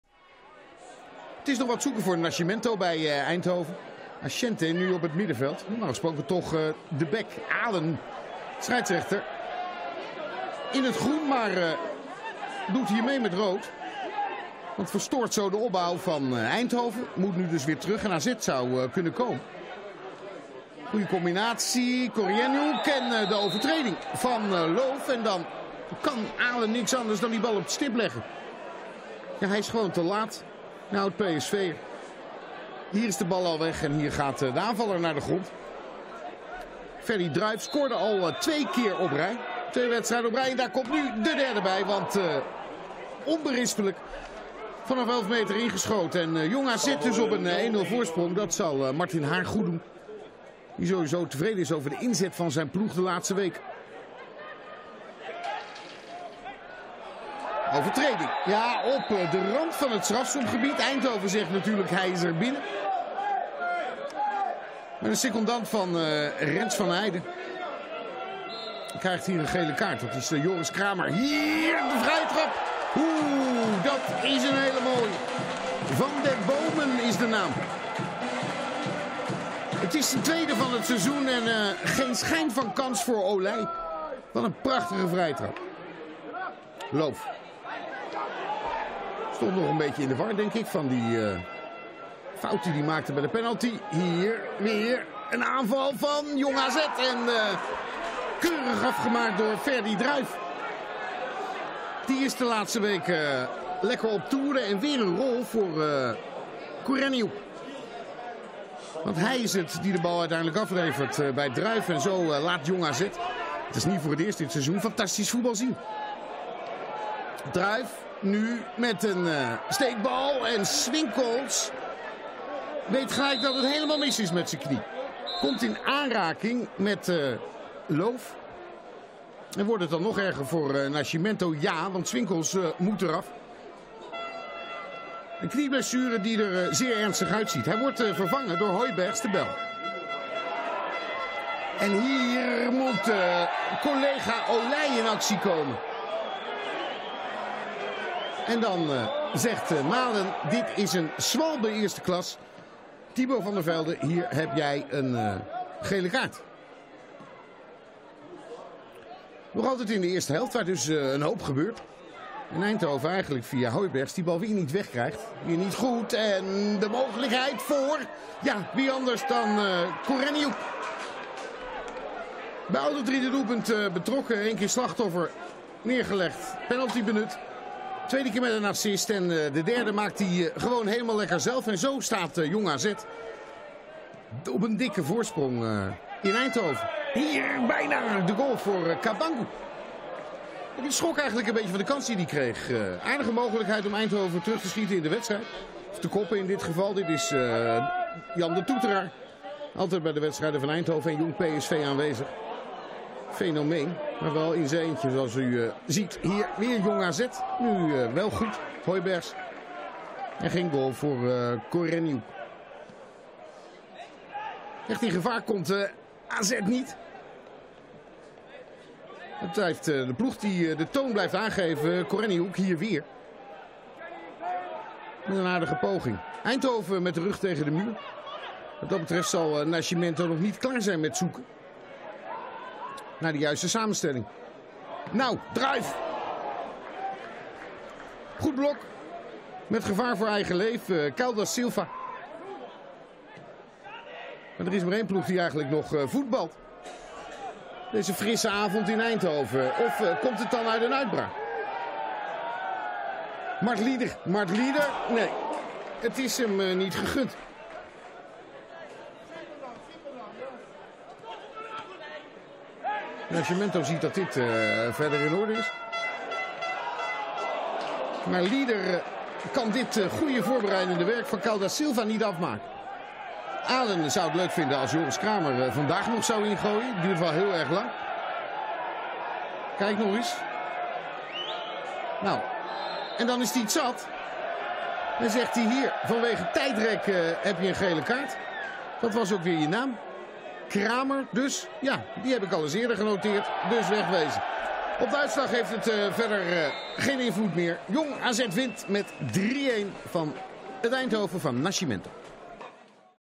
Goals FC Eindhoven - Jong AZ
Samenvatting van het Jupiler League-duel tussen FC Eindhoven en Jong AZ (1-3).
Scoreverloop: 6. Druijf 0-1 (pen.), 15. Van den Boomen 1-1, 21. Druijf 1-2, 45+2. Koreniuk 1-3.